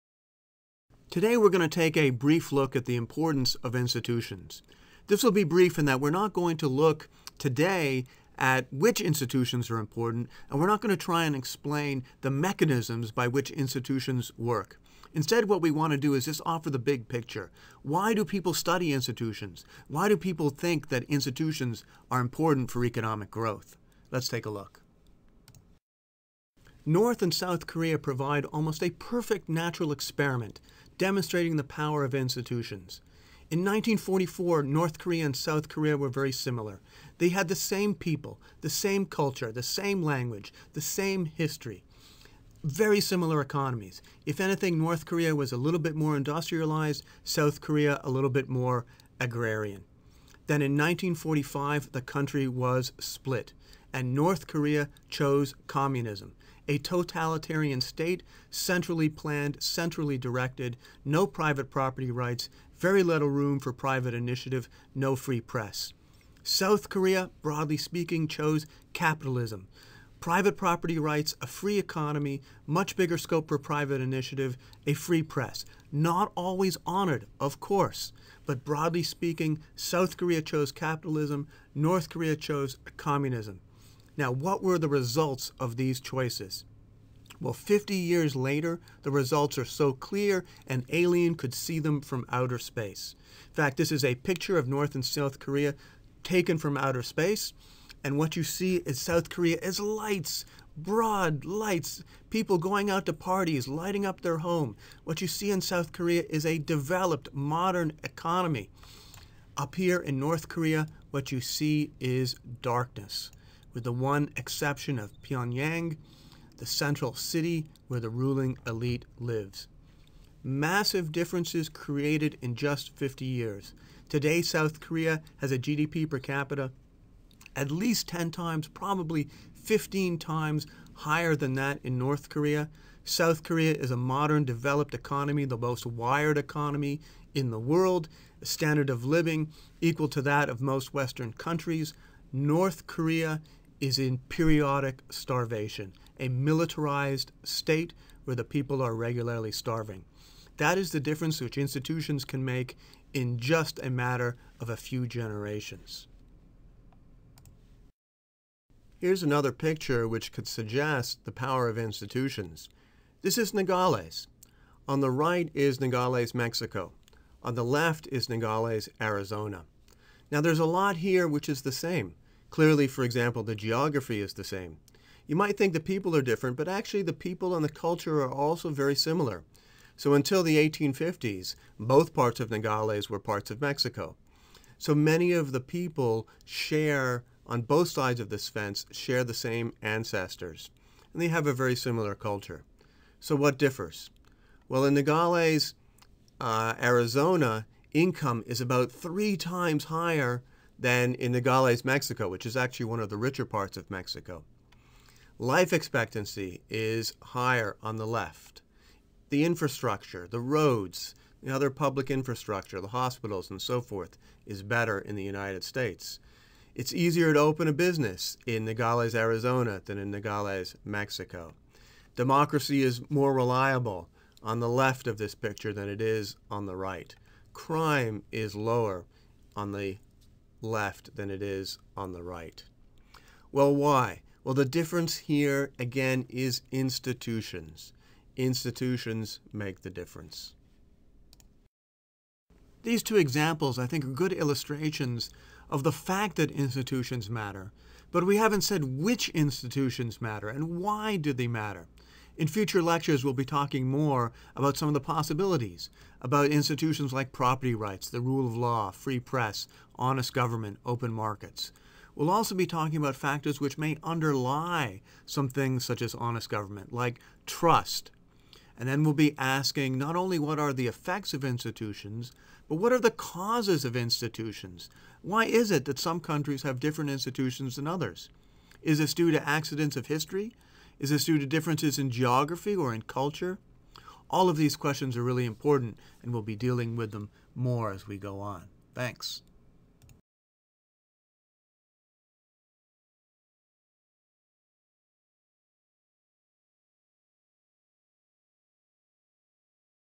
The Importance of Institutions (Brief)
The power of institutions illustrated. But what causes institutions? Can we change institutions?